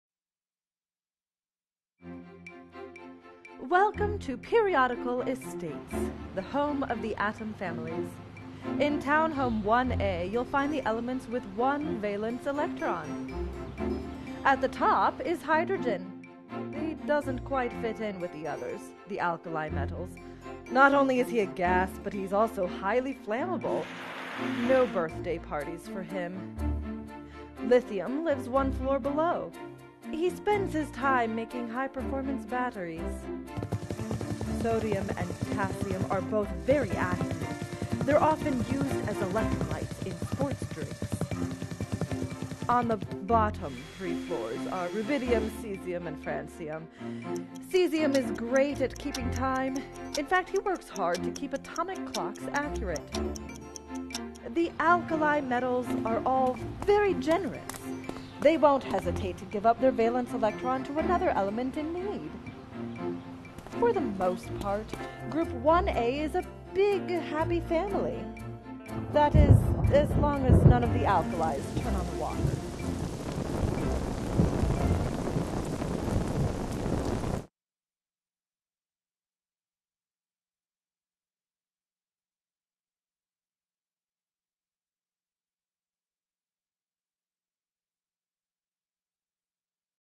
Periodical Estates: Alkali Metals
"Periodical Estates: Alkali Metals" is a favorite segment from Bill Harmon’s Chemistry 4th edition course. In this first Periodical Estates segment, students learn about the Group 1A alkali metals. Other segments in this series cover alkaline earth metals, boron family, carbon family, halogen family, nitrogen family, noble gas family, oxygen family, and transition metals.